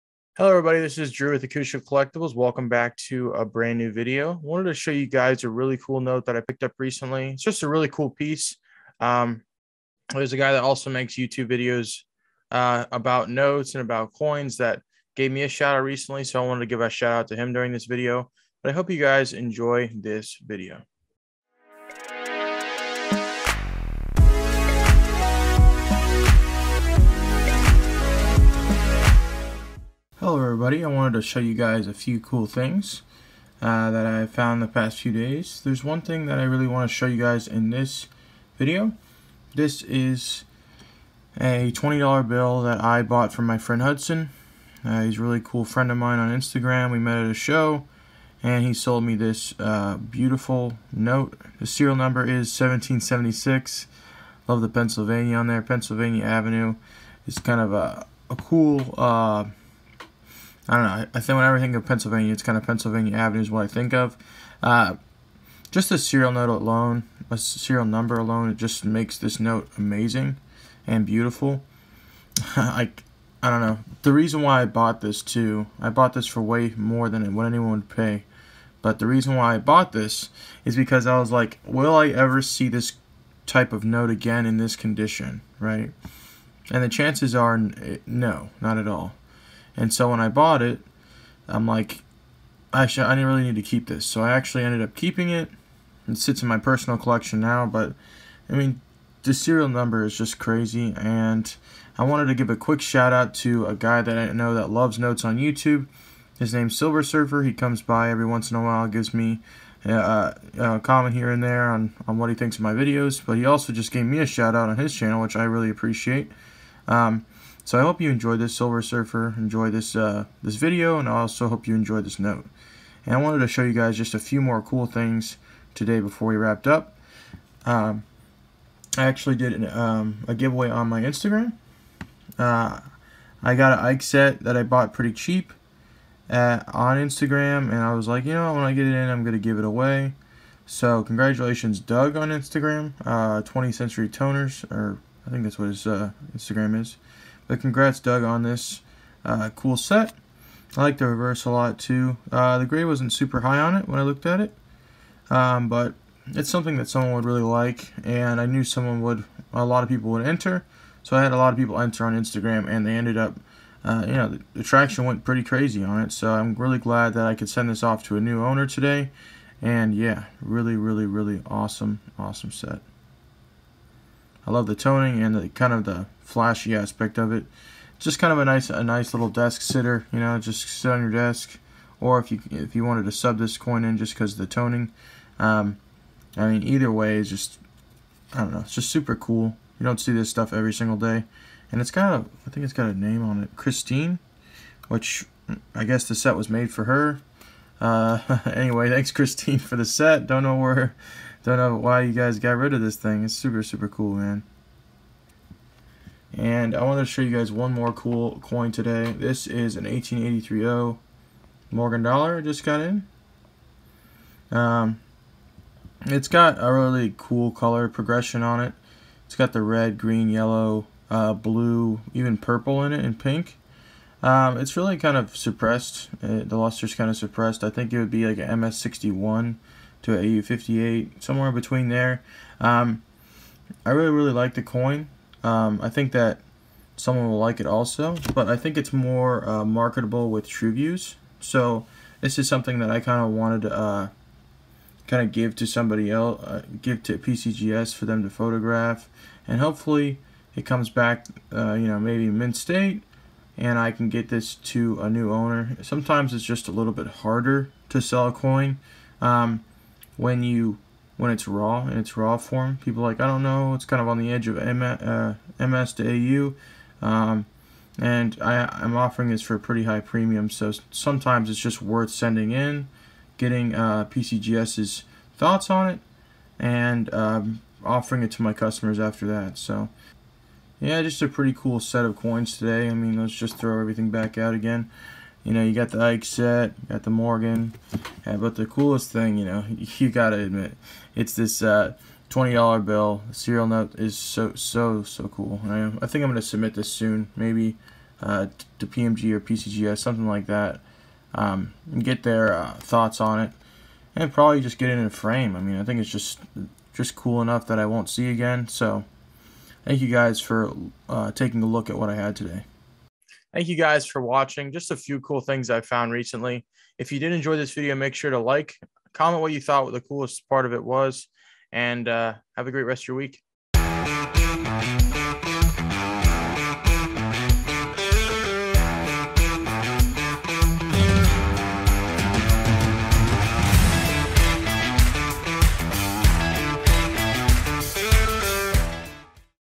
A coin dealer adds a RARE note to his collection | 1776 Serial Number!
Found this amazing note at the recent Summer FUN coin show in Florida. It was an absolutely sweet piece that I couldn't pass it up! Thank you Hudson for the 1776 note!

Thank you SilverSurfer for being a great fellow youtuber we doesn't mind supporting smaller creators like myself!

Drew Haddock